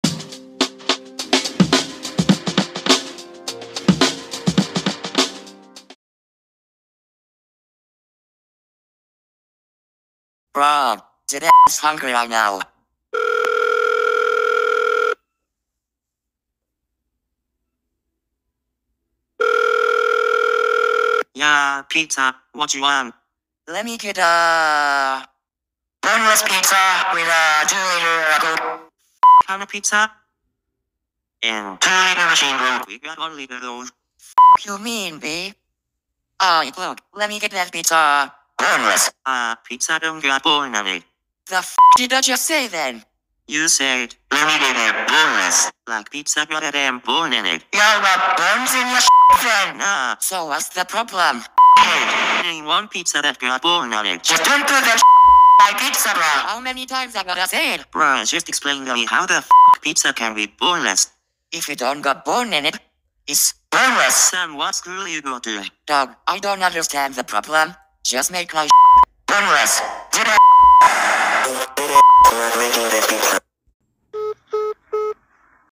[Plush Talkloid] Boneless Pizza
I know this is a very old meme but I wanted to make a plush talkloid of it. Also this video and future plush talkloids aren't part of the main plush series on the channel. The plush talkloids will be their own thing.